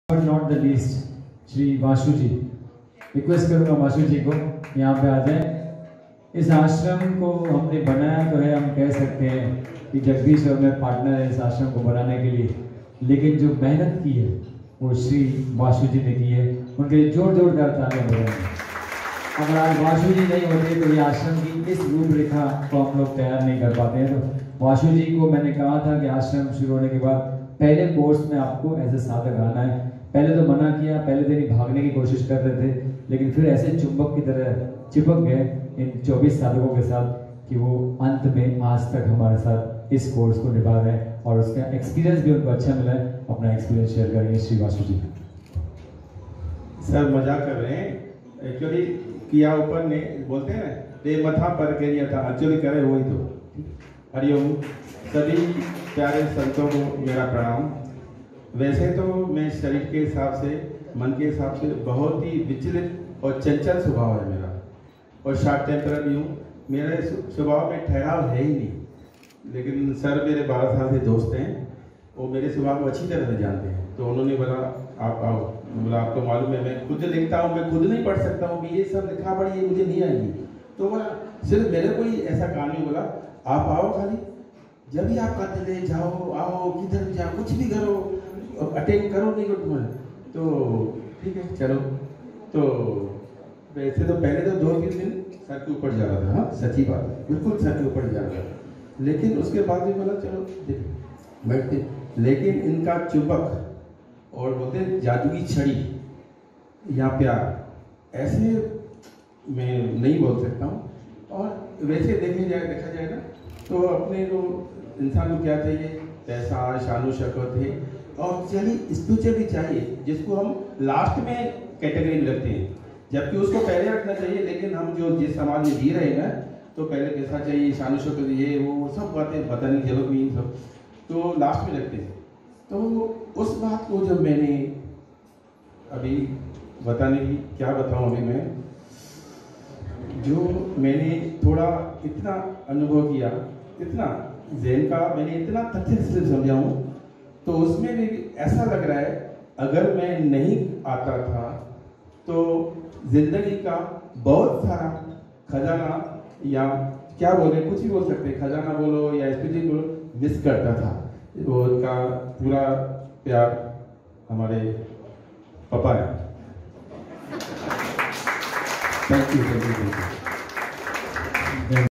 1st Ever First Zen Yoga Residential Course Experience(Shri Vashu Khemchandani)16-01-21 till 24-01-21
Shri Vashu Khemchandani sharing his Expectations, Experiences, Views about the Zen Yoga Residential Course at Dr. Saherji's International Zen Yoga IESM Foundation, Badlapur.